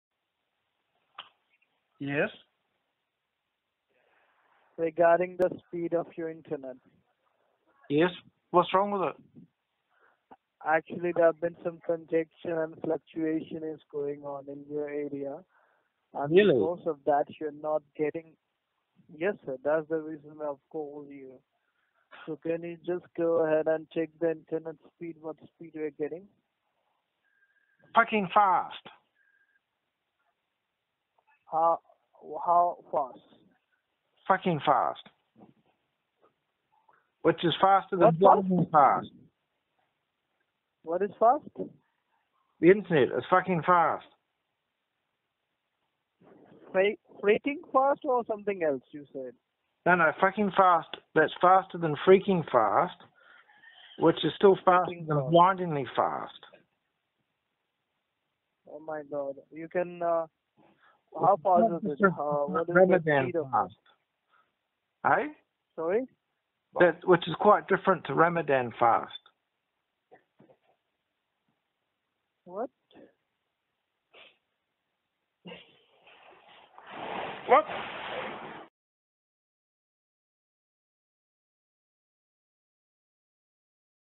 Spark 23 Aug 2019
Those Indian Scammers phoned me again.